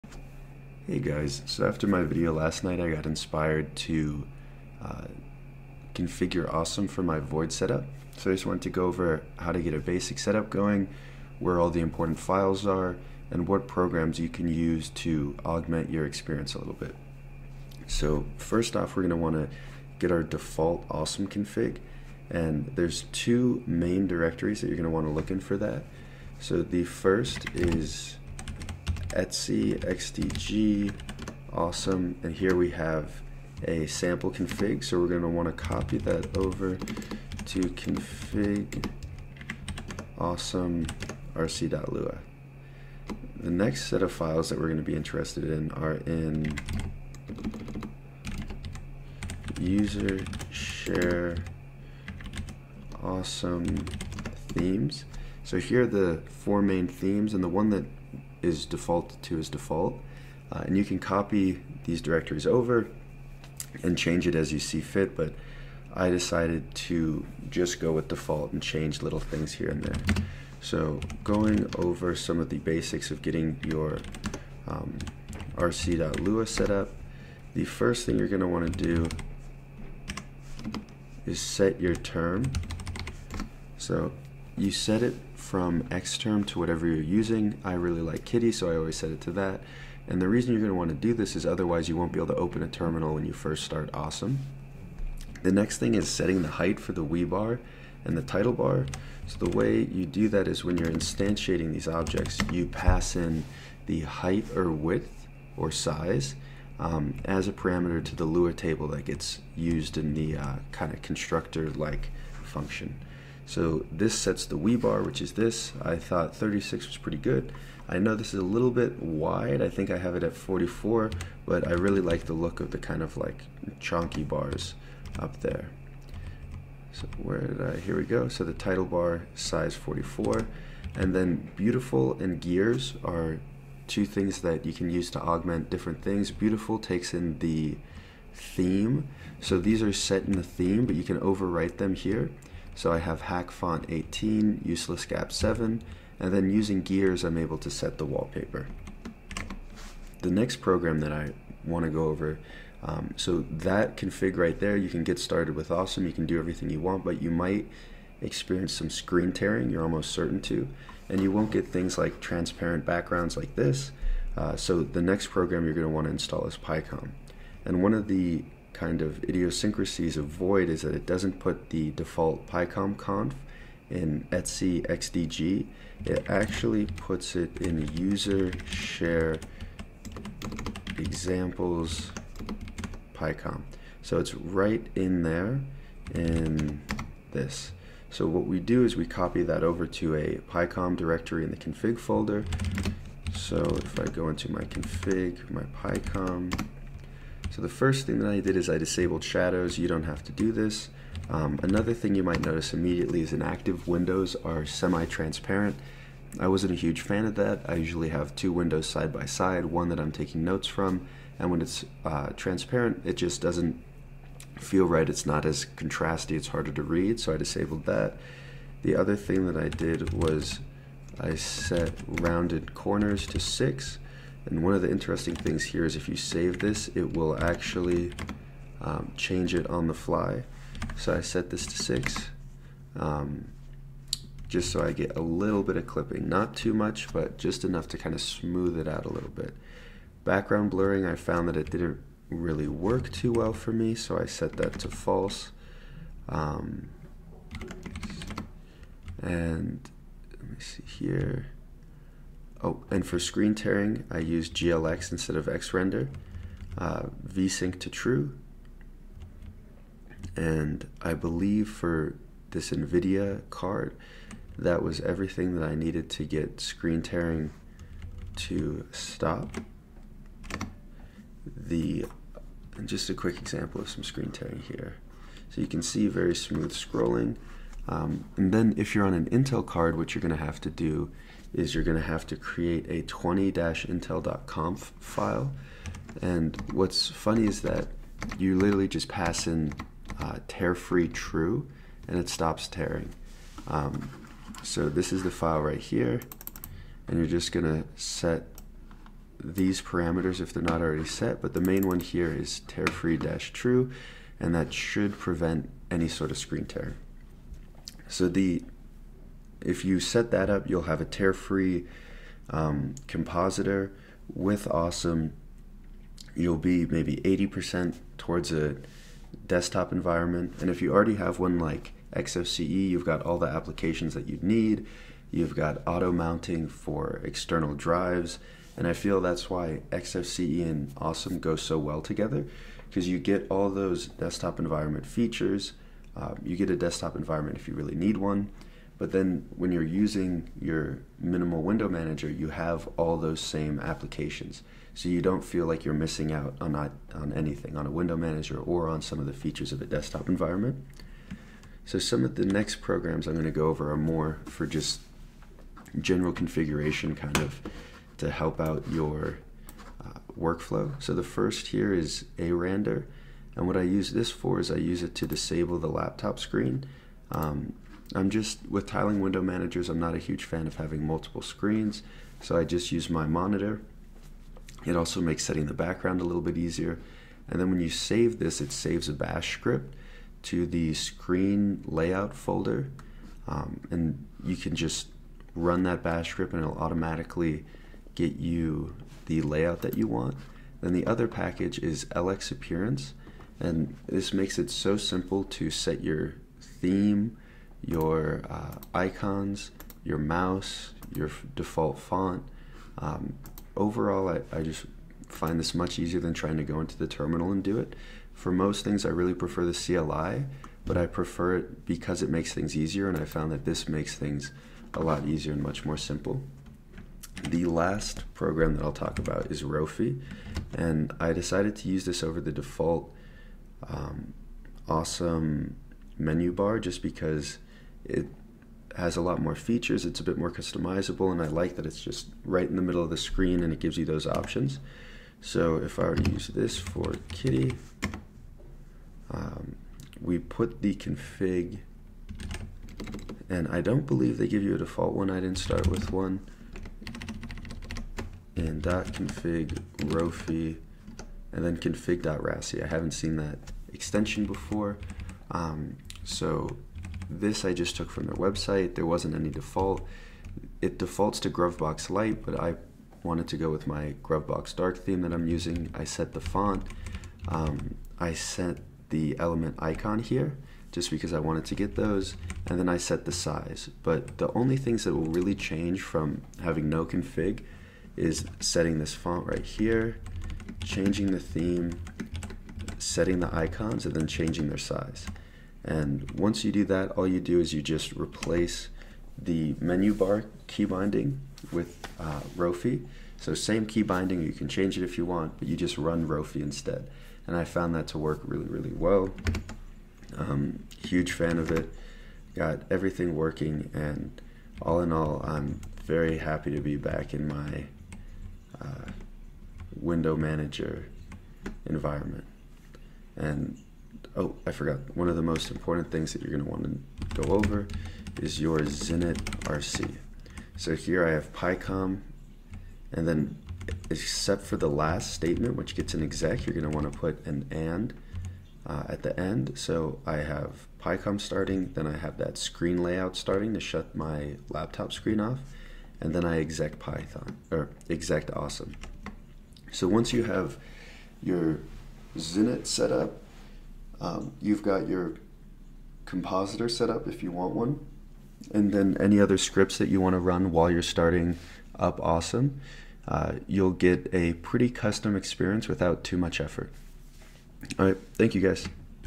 Awesome(WM) Void Linux setup a match made in FOSS heaven
After yesterday's video I decided to cobble together a bare bones awesome config with picom and some simple theming.
some helpful links:
- tearing: